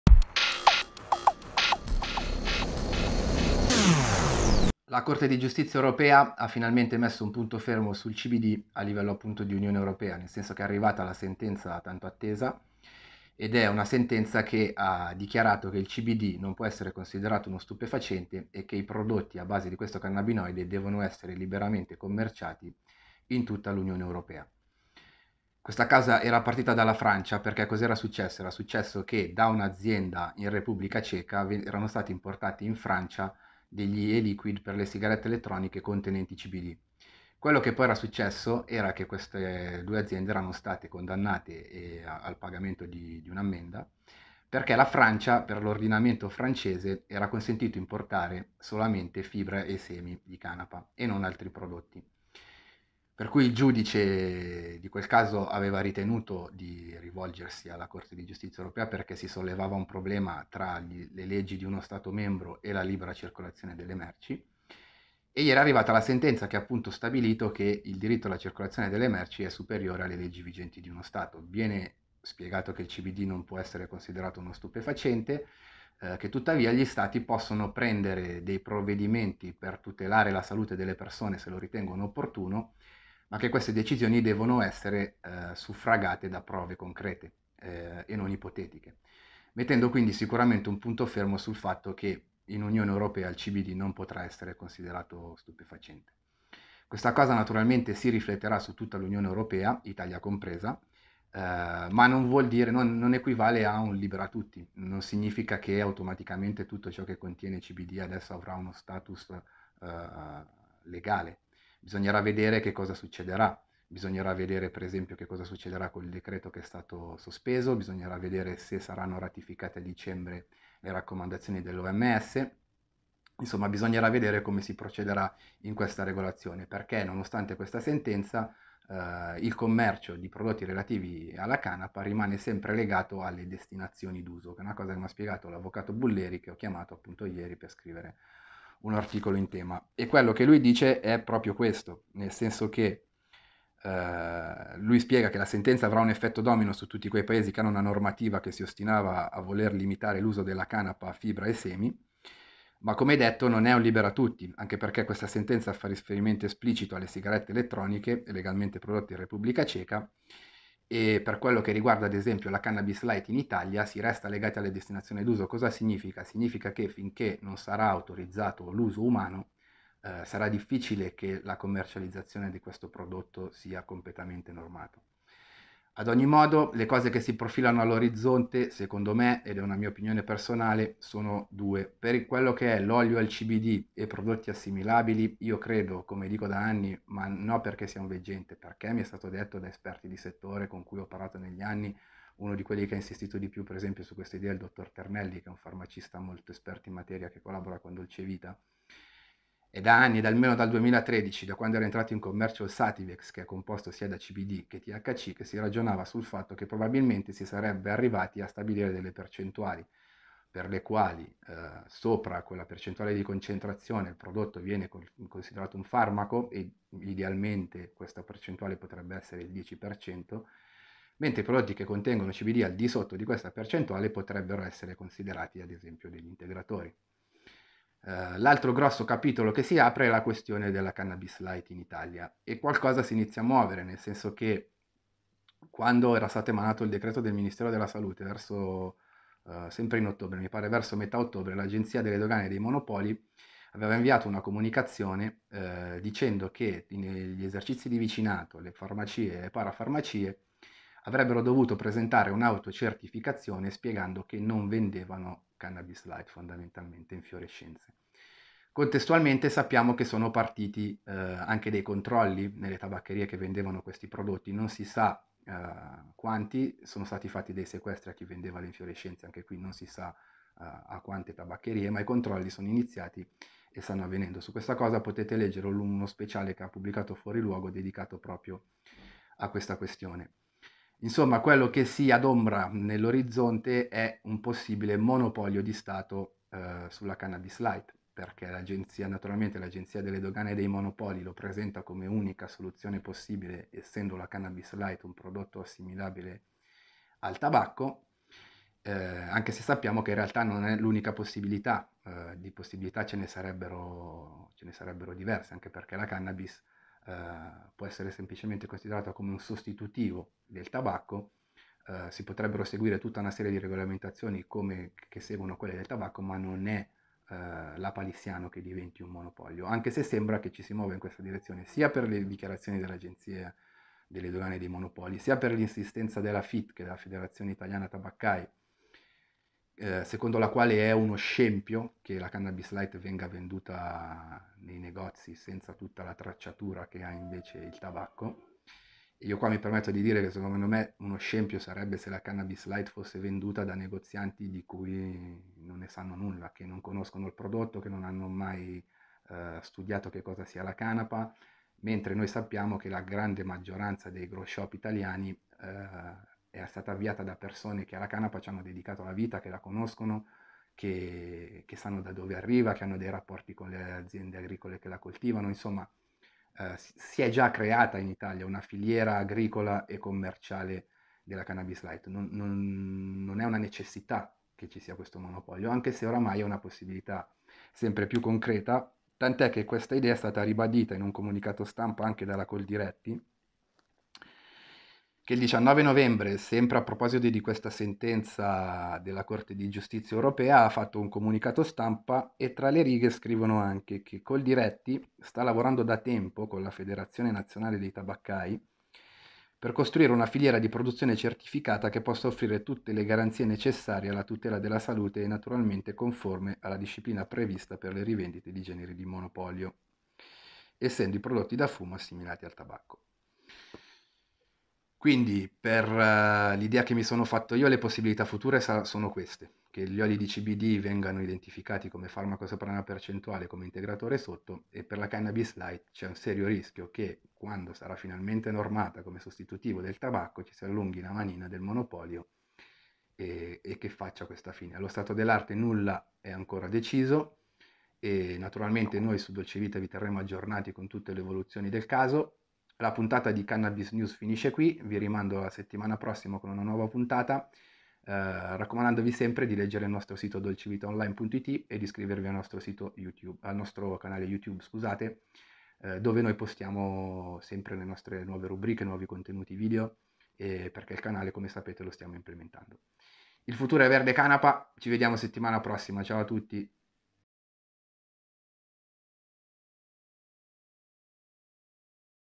Dalla sentenza europea sul CBD al possibile monopolio per la cannabis light
Dalla sentenza della Corte di giustizia europea fino a ipotizzare il futuro di oli al CBD e cannabis light in Italia.

Seguiteci..